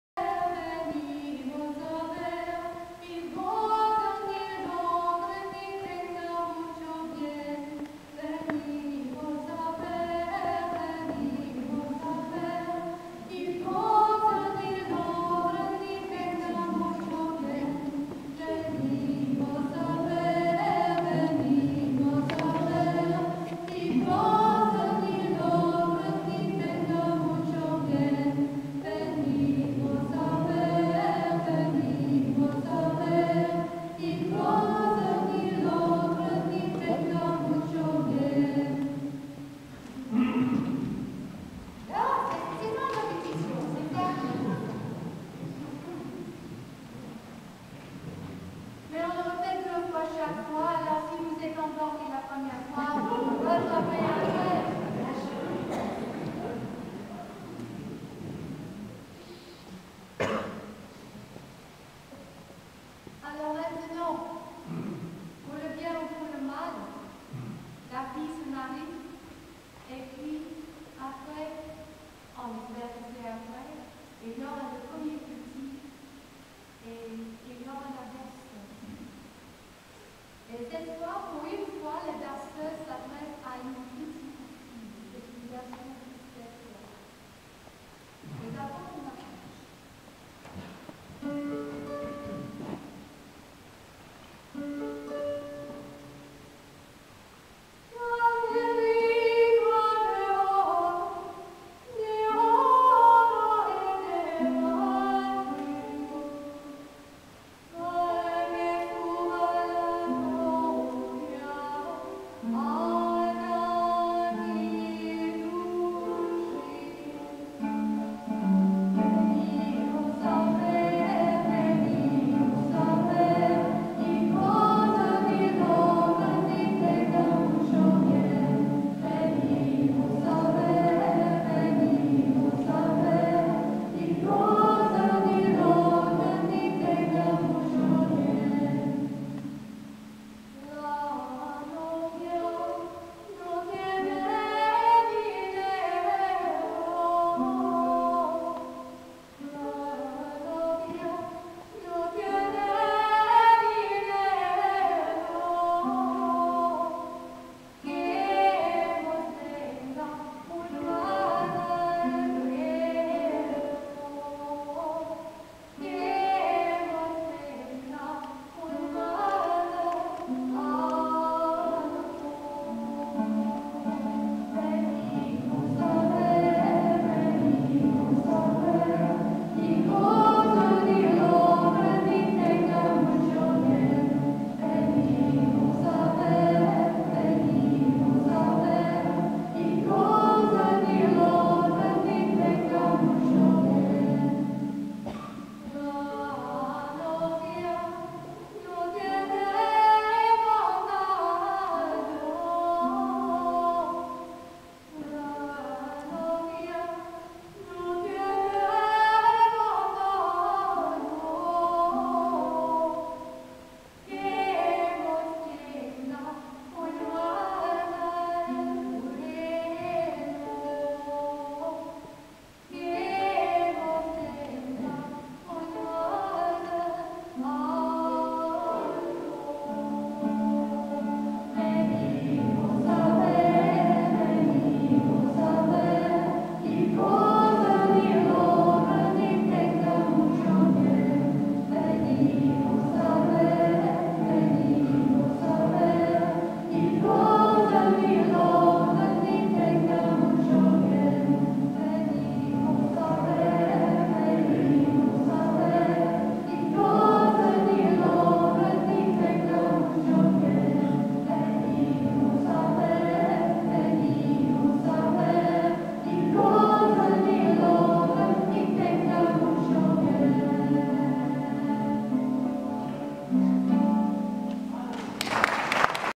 Concert pour la chapelle de fourcheval
La très dynamique association pour la restauration de la chapelle de Fourcheval n'a pas ménagé sa peine pour proposer un concert de qualité : la chorale "comme un accord", le groupe "les Druginaires" et l'Union Musicale Marsacoise.